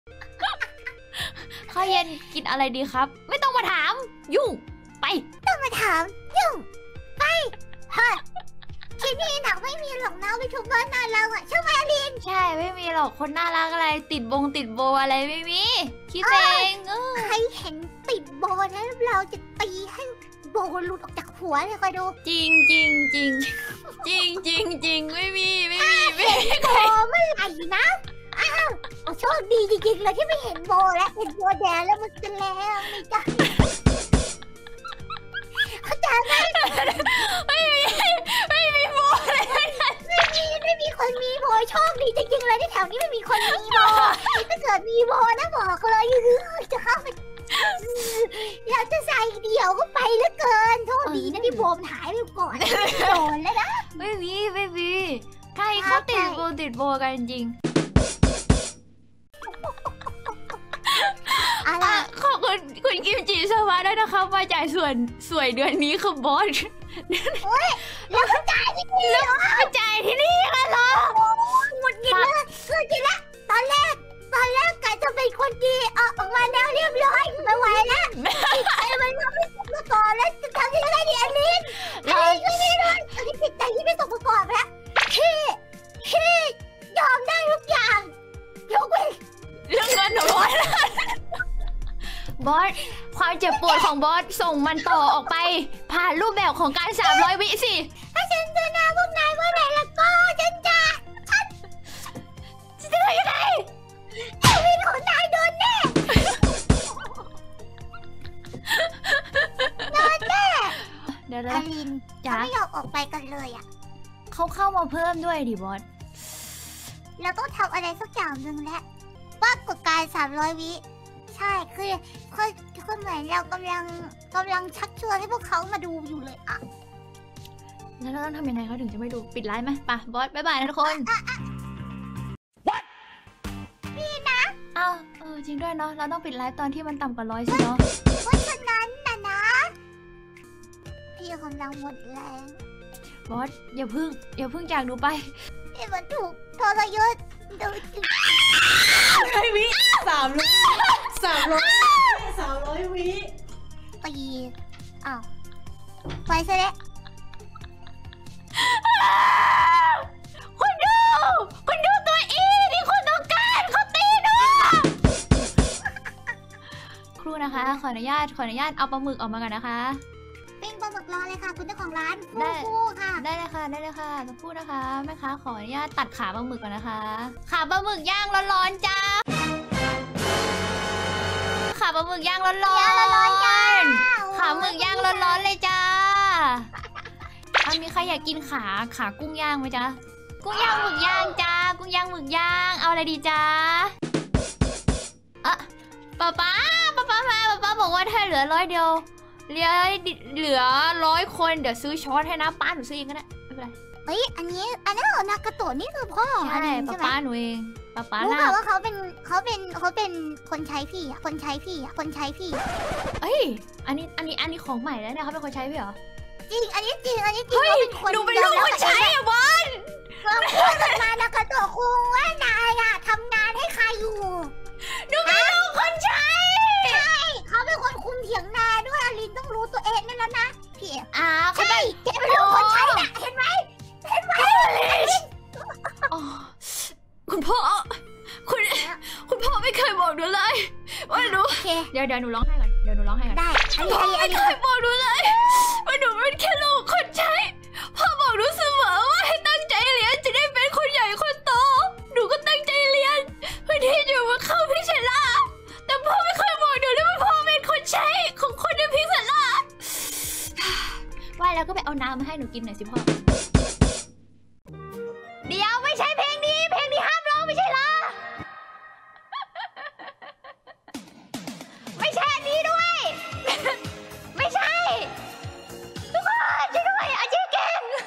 [Vtuber]Hi-light อาลิน เป็นลูกคนใช้
ความลับถูกเปิดเผยแล้ว จากปาก ปะป๊าของอาลิน!!

ตามไปตำและดูไลฟ์ย้อนหลังได้ที่นี่